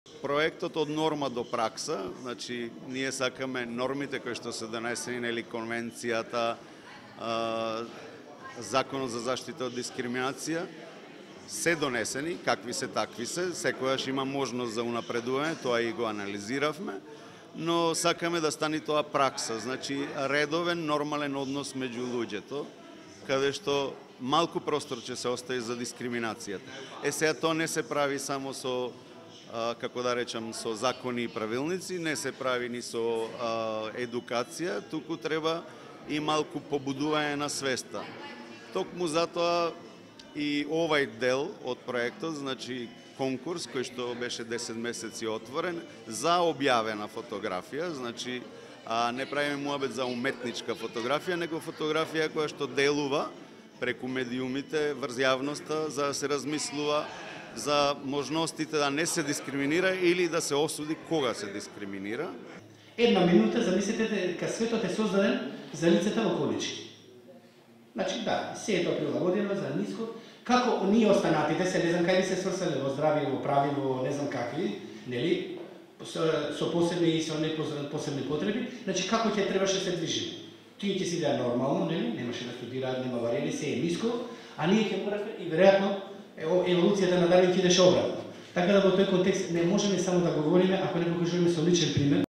Awards for the media and photographs
In order to raise their awareness of the benefits of diversity in all areas of society considering the equality and non-discrimination Polio Plus has conducted a photo and poster competition  "Focusing Discrimination".
This action was a part of the project from "Legislation to practice"  supported by the European Union Programme for Employment and Social affairs and inclusion -- PROGRESSS (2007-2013). The exhibition of the best photos and posters took part on the International Day for human rights - 10th December 2013.

The project was implemented by Polio PLus - movement against discrimination; MLSP - Ministry for Labor and Social Policy; Commission for protection against discrimination - CPD; Macedonian Center for International Cooperation - MCIC.

This video was part of the news covered by TV SItel